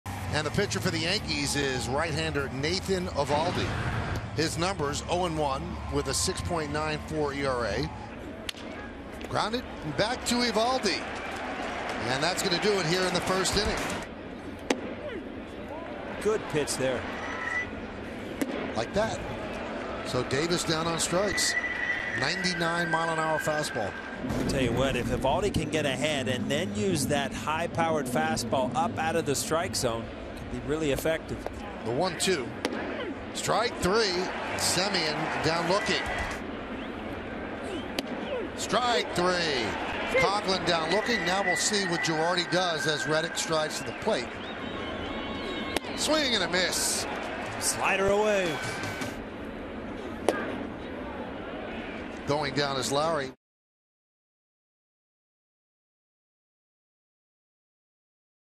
OAK@NYY: Eovaldi strikes out seven over six frames
Nathan Eovaldi strikes out seven over six quality innings, allowing three runs on eight hits and a walk